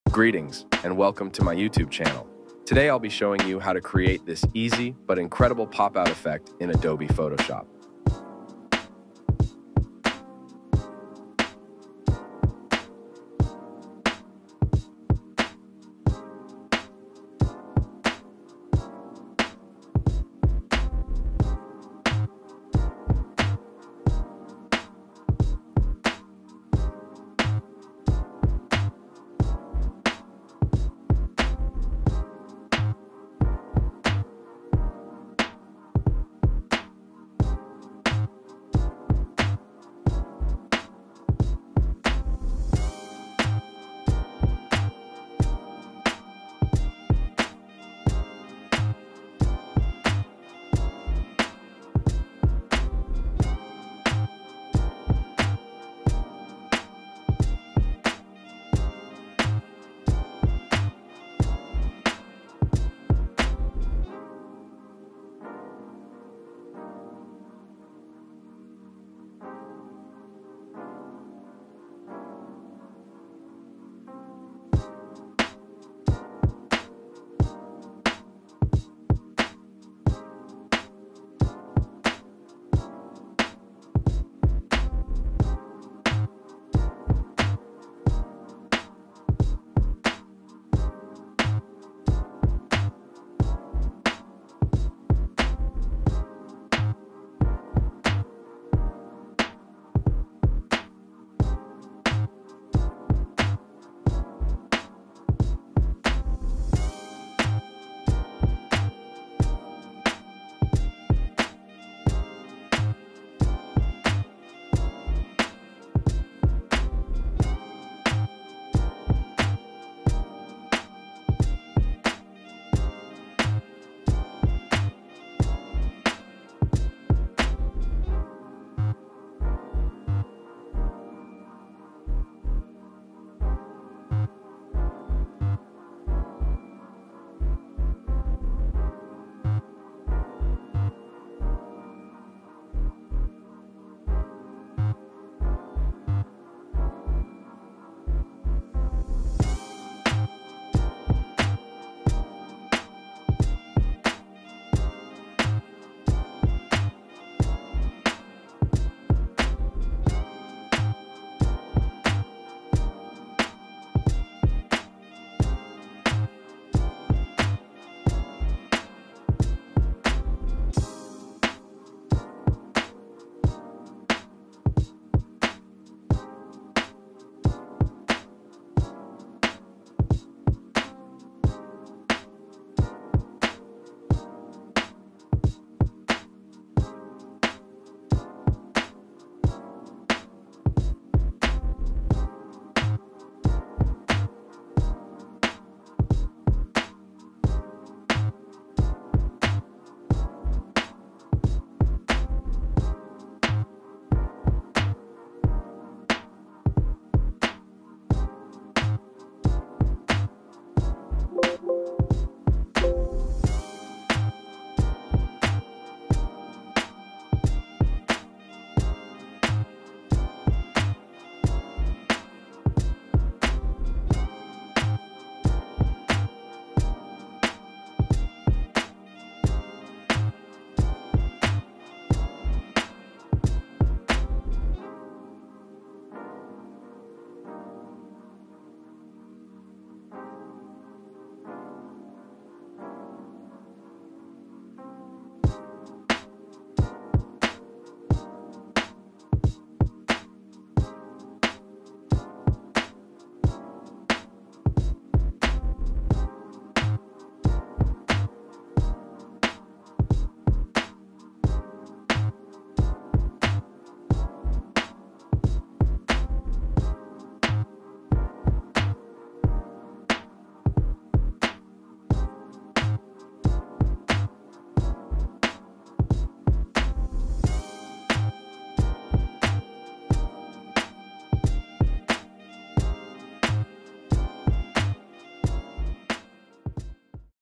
Making a Pop Out Photoshop effect
Greetings, everyone! on this occasion, I will be creating this pop-out effect using Adobe Photoshop.

Hello, in this video I will show you how to create a surreal pop out design in adobe photoshop, you can find all the images used in this video at the end of the description, if you like this type of content do not forget to support us with a like and share our videos .